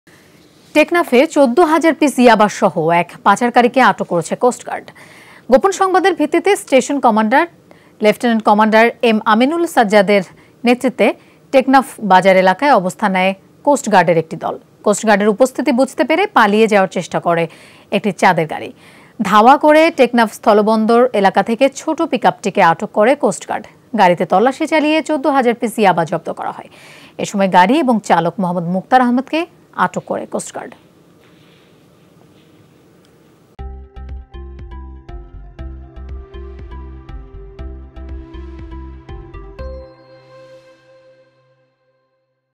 টেকনাফে ১৪ হাজার পিস ইয়াবাসহ পাচারকারী আটক | Yaba Arrest
টেকনাফে ১৪ হাজার পিস ইয়াবাসহ এক পাচারকারীকে আটক করেছে কোস্টগার্ড।